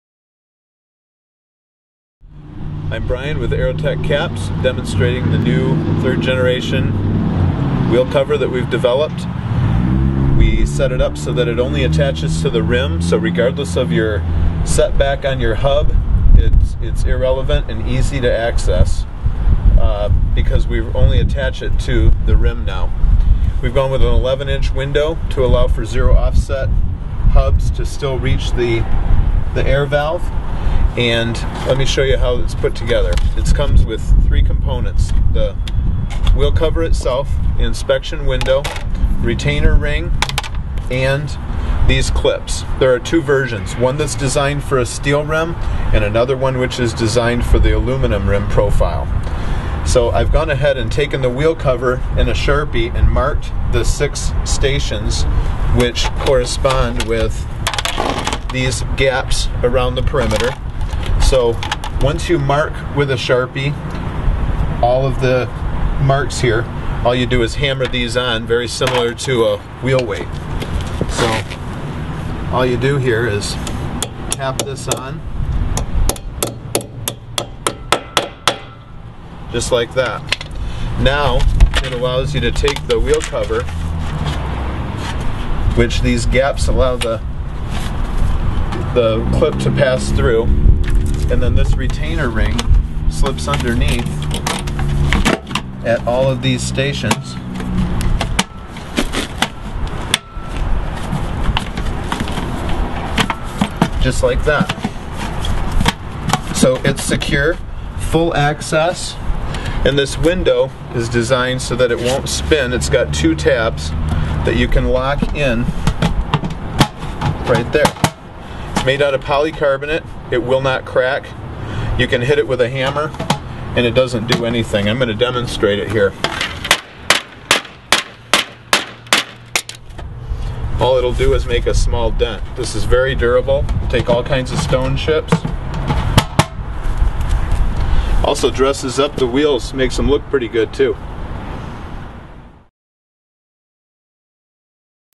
AeroTech Caps Installation Video — Steel Wheels
Installing AeroTech Caps aerodynamic wheel covers on a semi truck with steel wheels.